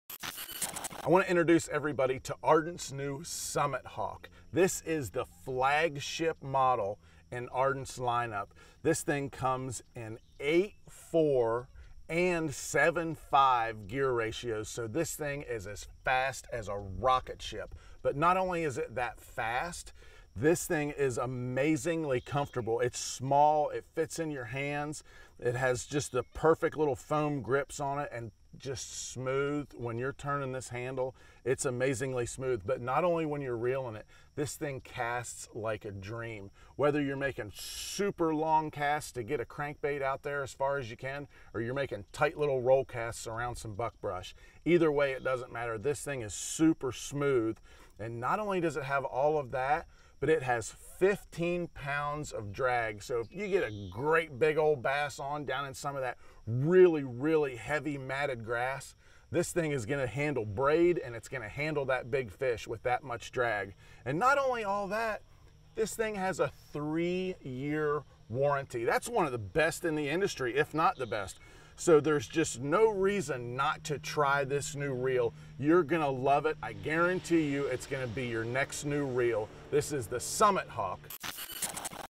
Ardent Summit Hawk Baitcasting Reel
Major League Fishing Commentator and Ardent Pro JT Kenney discusses the features and benefits of the Ardent Summit Hawk Baitcasting Reel. Get yours at ardentoutdoors.com